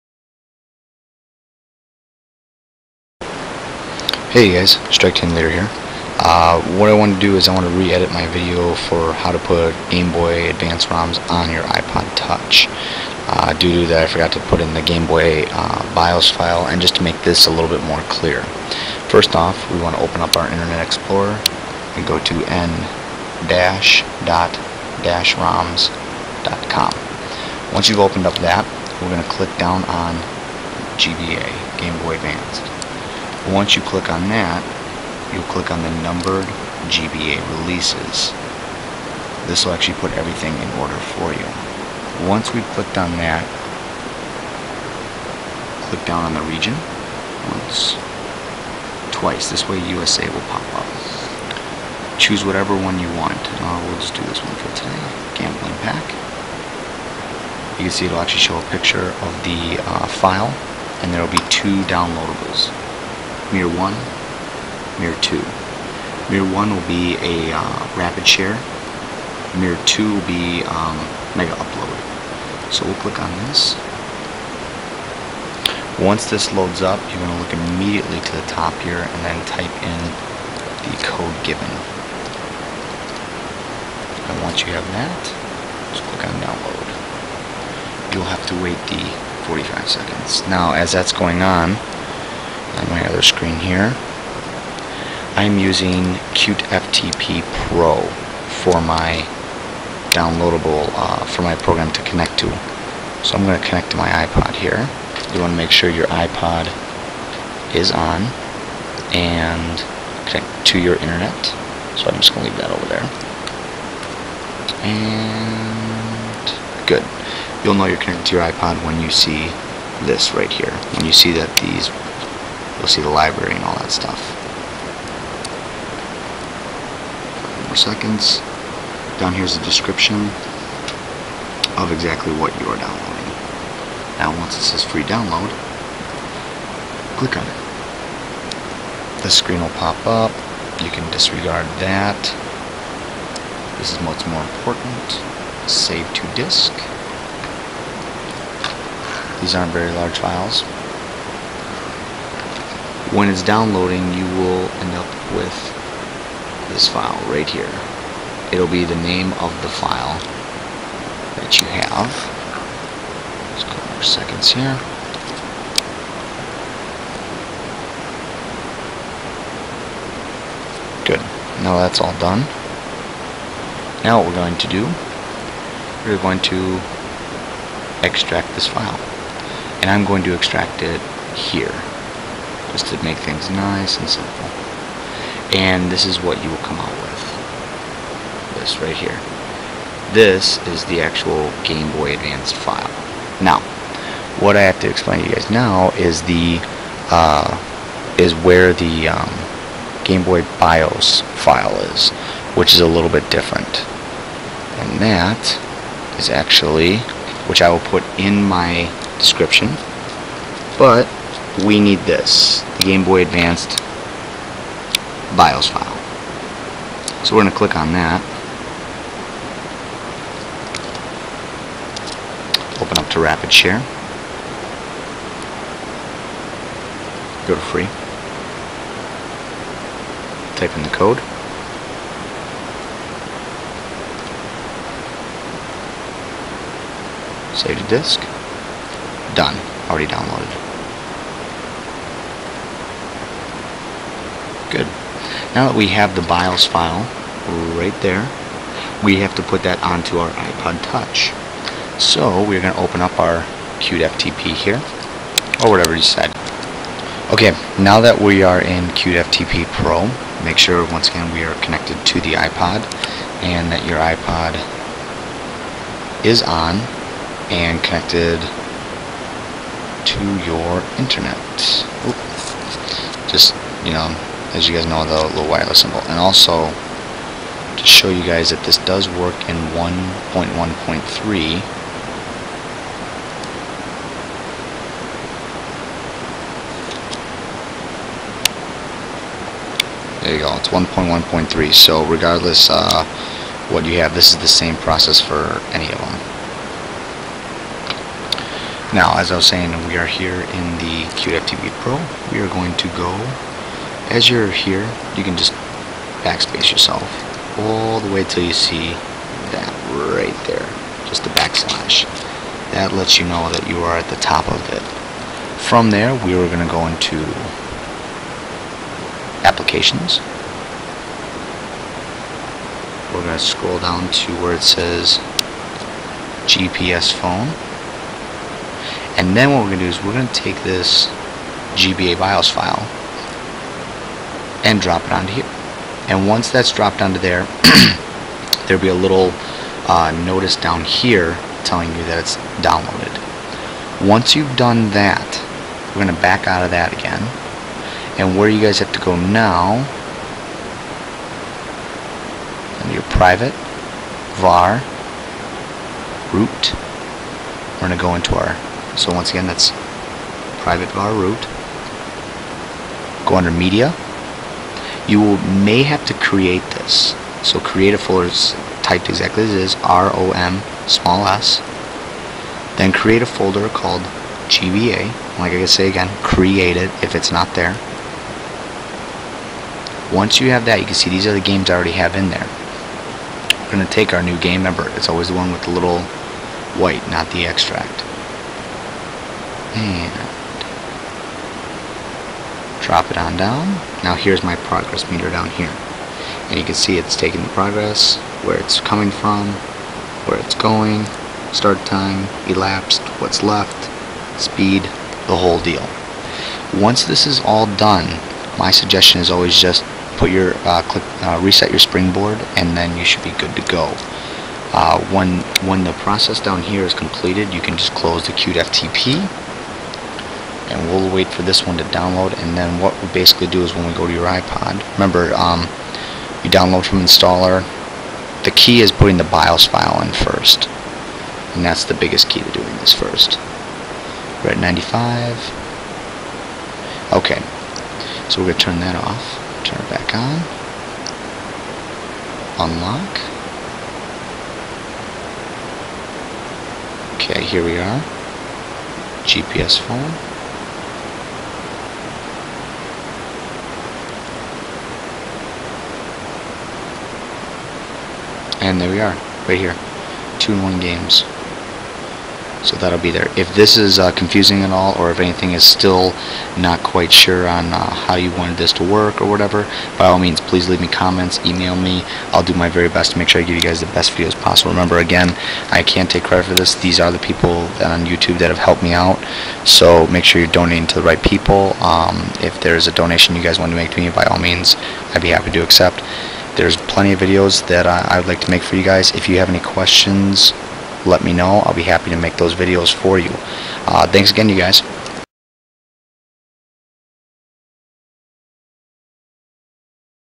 GBA ROMs Installed on iPod Touch with Computer.
This is an older method but I believe to be safer for the fact you wont have a strange source on your iPod. Thanks again for watching.

(Install in the applications/gpsphone folder and put file in there.)